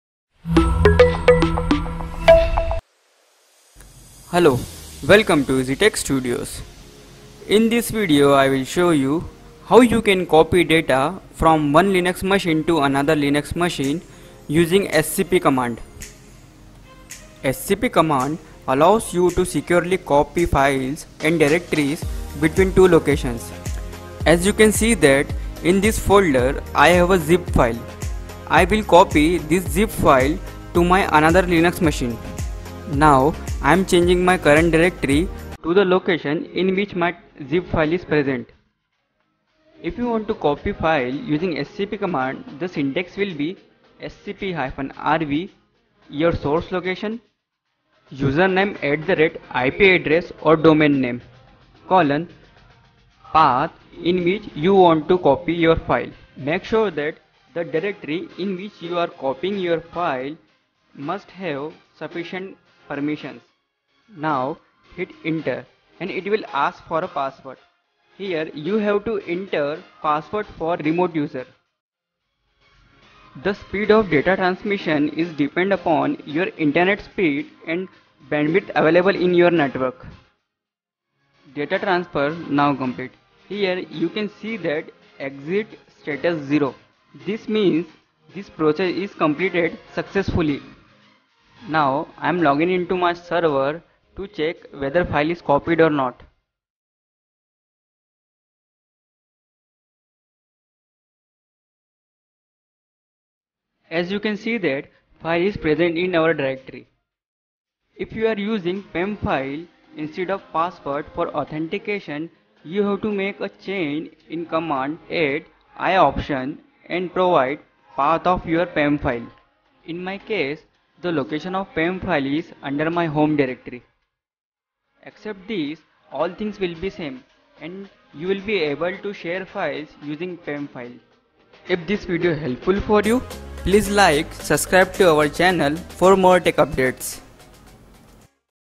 How to Copy Files from One Linux Server to Another | SCP Command For Beginners
In this video, I have covered How to Securely Transfer Files Between Servers.